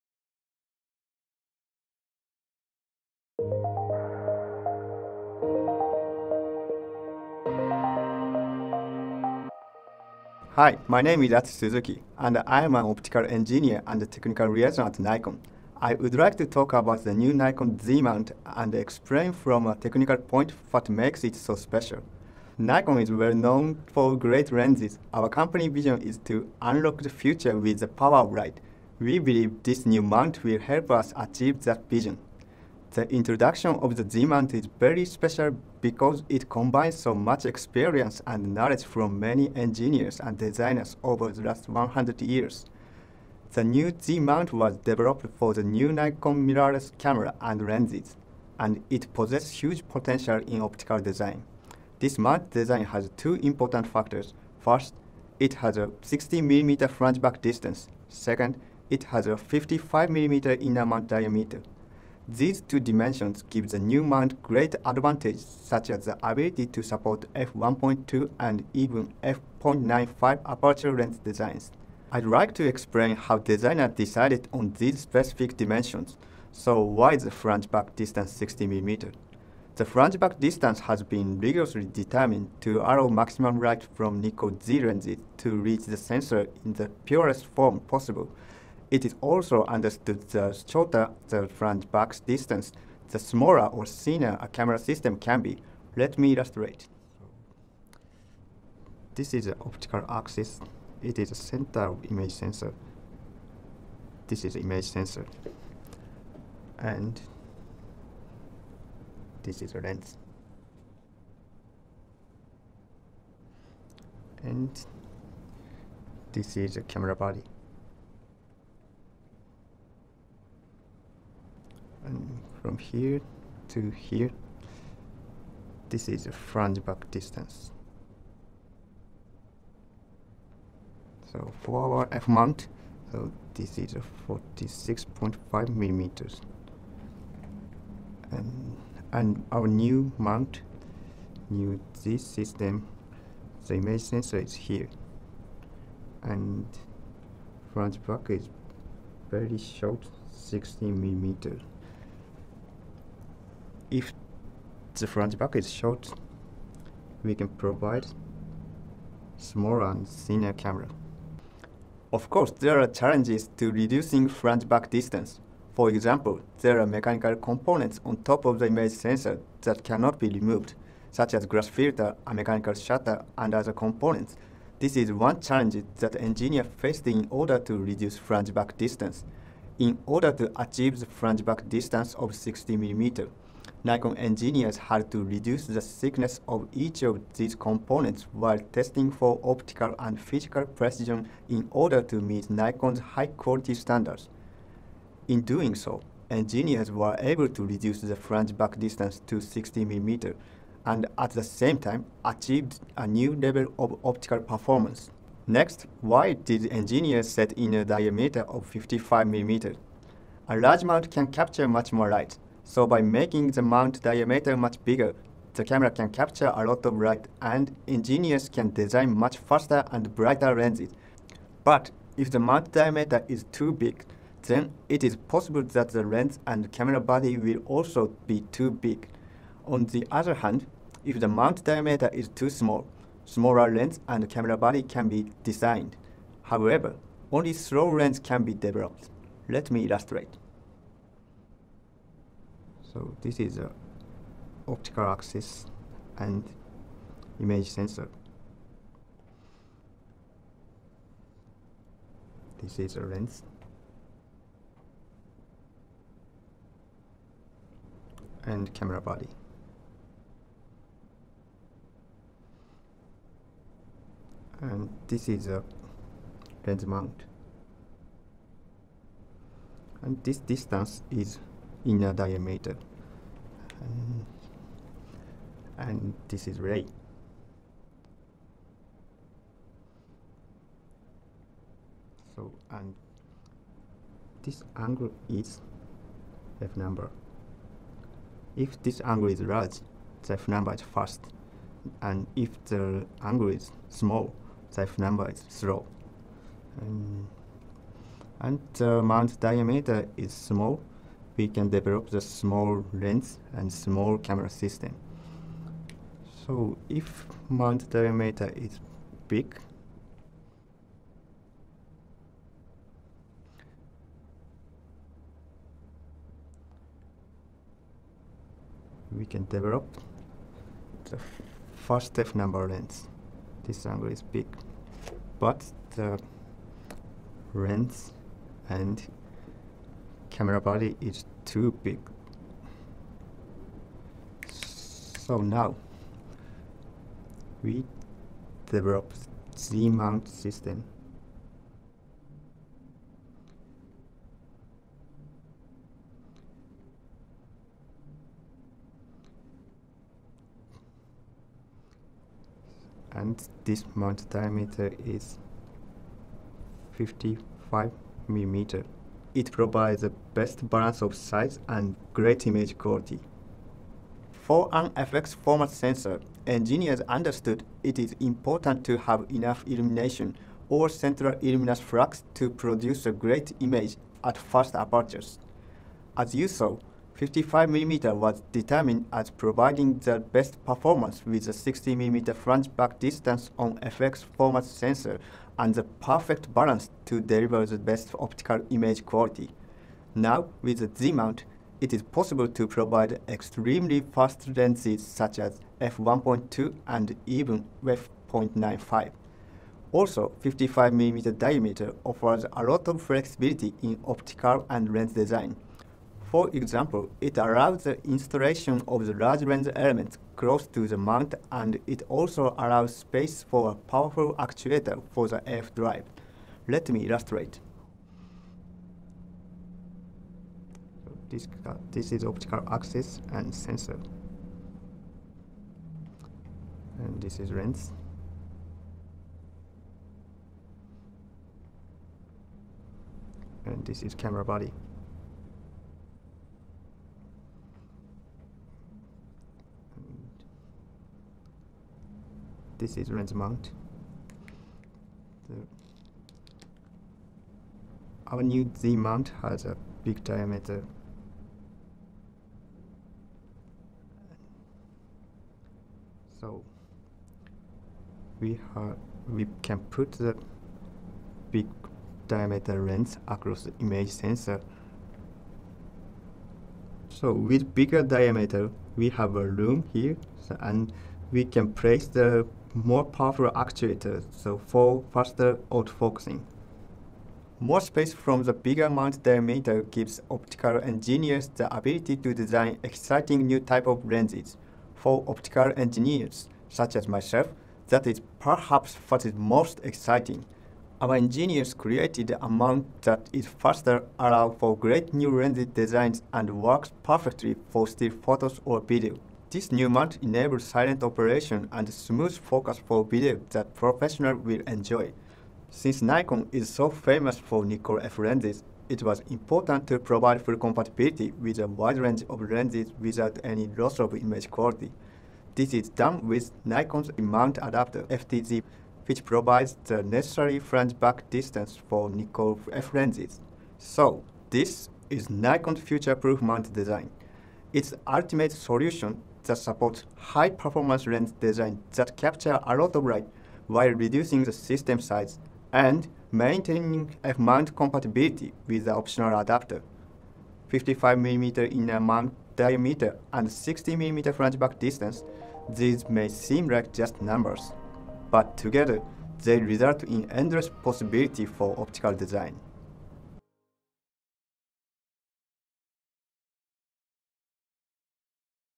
La Montura del Sistema Nikon Z | Estructura y diseño
Atsushi Suzuki, Ingeniero de Ópticas y Coordinador Técnico de Nikon, ilustra en este vídeo la estructura y el diseño de la nueva montura del sistema Nikon Z, y nos explica el porqué de sus ventajas.

Descubre más sobre la Montura Z, diseñada para el máximo rendimiento óptico